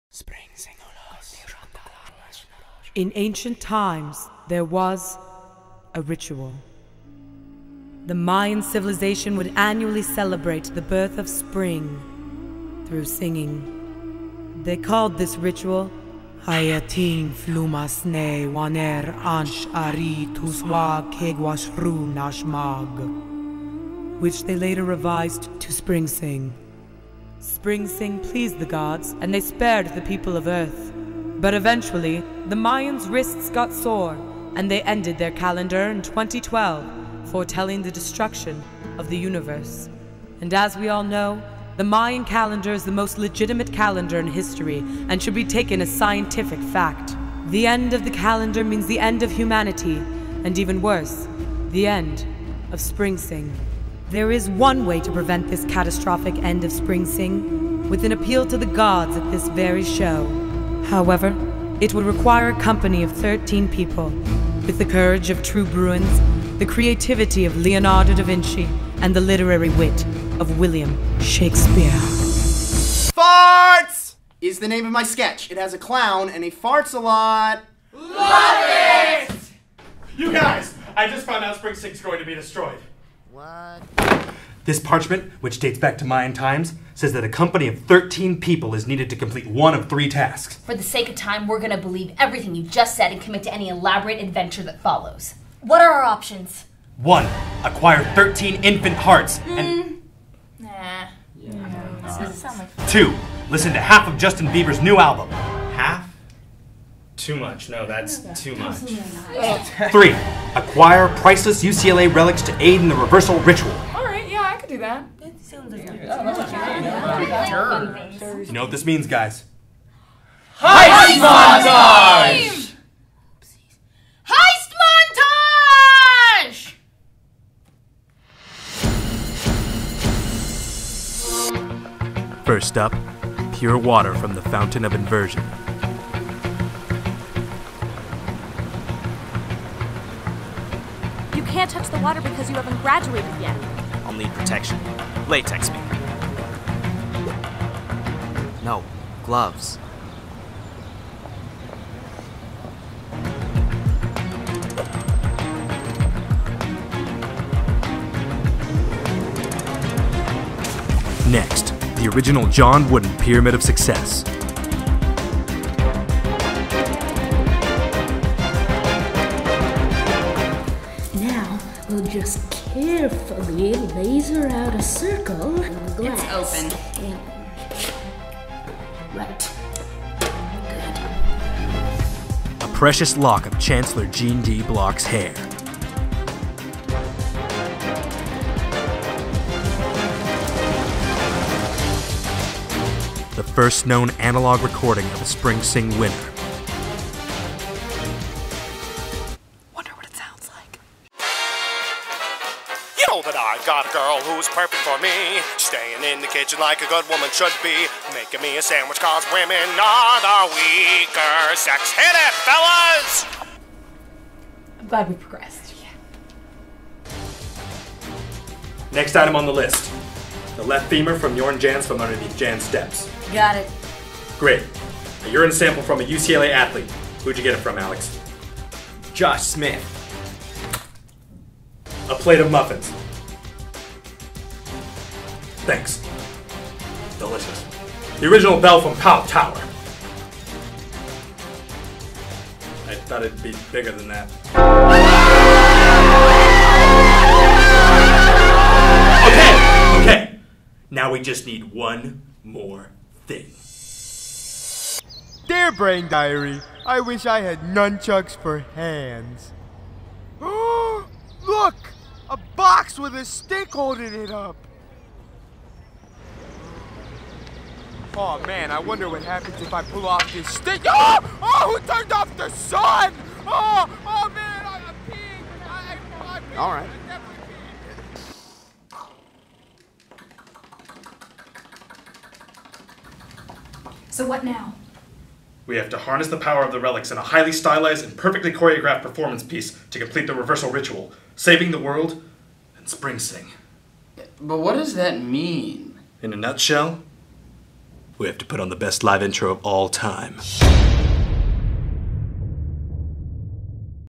Intro Video (Spring Sing 2012)
The fate of Spring Sing and the world lies in the hands of 13 company members.

Directed by Nick Smith

Animation by Brendan Rice

Spring Sing Company 2012